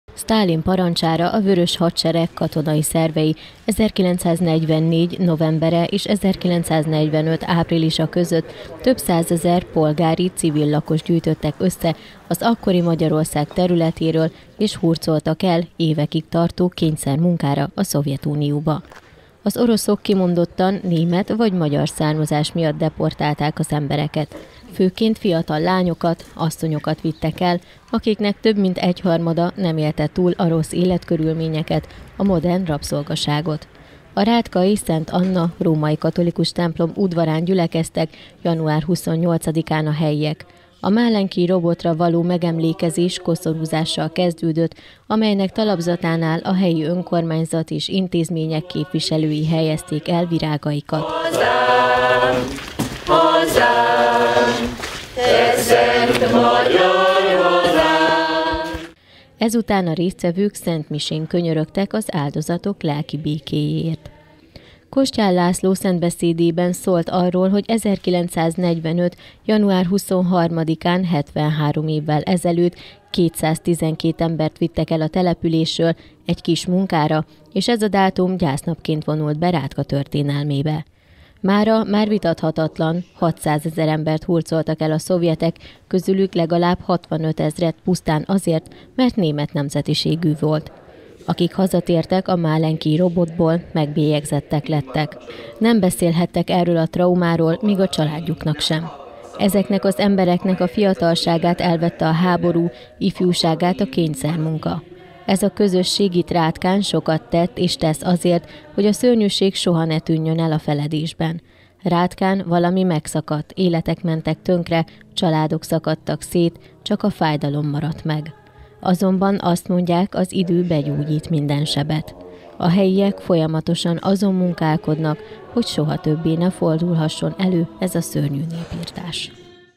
EMLÉKEZÉS A RÁTKÁRÓL ELHURCOLTAKRA
A rátkai Szent Anna római katolikus templom udvarán gyülekeztek január 28-án a helyiek a málenkij robotra elhurcoltak emlékünnepségére. Az esemény az emlékmű koszorúzásával kezdődik, amelynek talapzatánál a helyi önkormányzat és intézményei helyezték el koszorúikat, virágaikat. Ezután a részvevők a templomban szentmisén könyörögtek az áldozatok lelki békéért. Kostyál László szentbeszédében szólt arról, hogy 1945. január 23-án, hetvenhárom évvel ezelőtt kétszáztizenkét embert vittek el a településről „egy kis munkára”, így ez a dátum gyásznapként vonult be Rátka történelmébe.